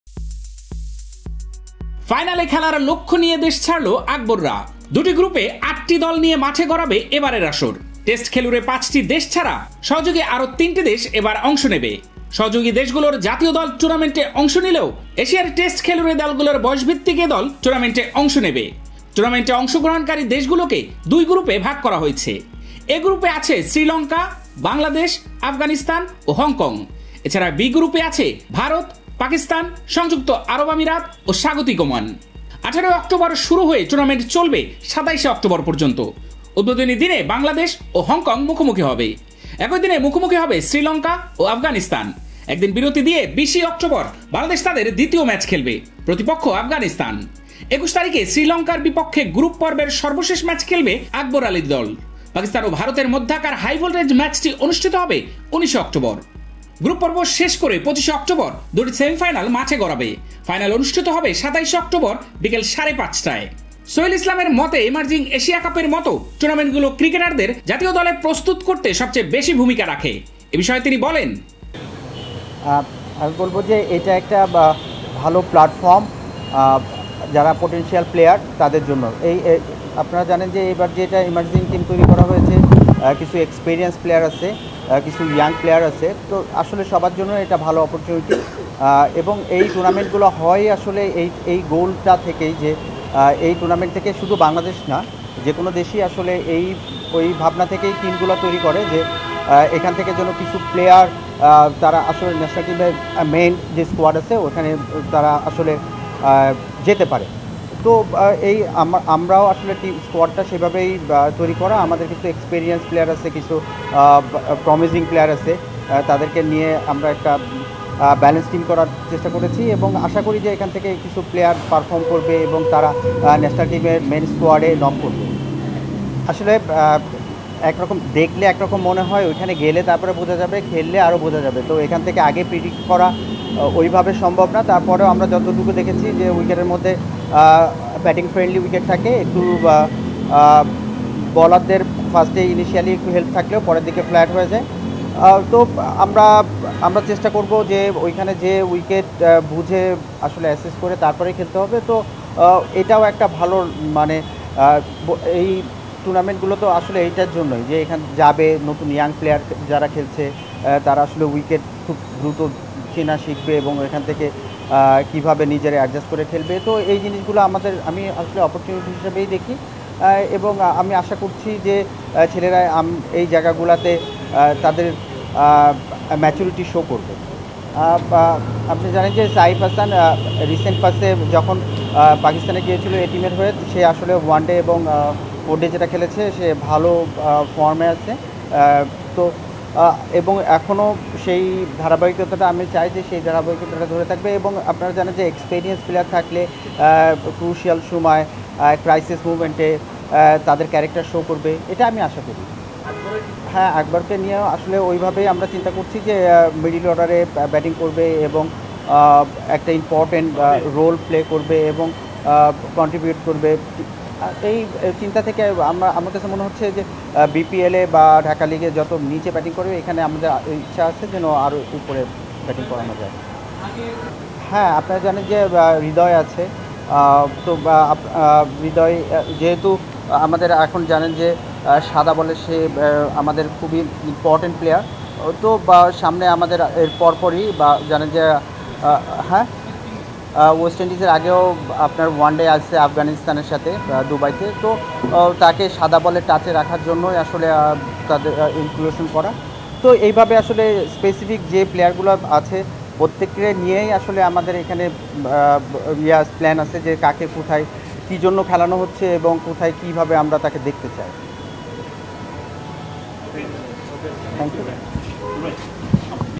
ফাইনালে খেলার লক্ষ্য নিয়ে দেশ ছাড়লেন হৃদয়-আকবররা।
Address:
Cricket Today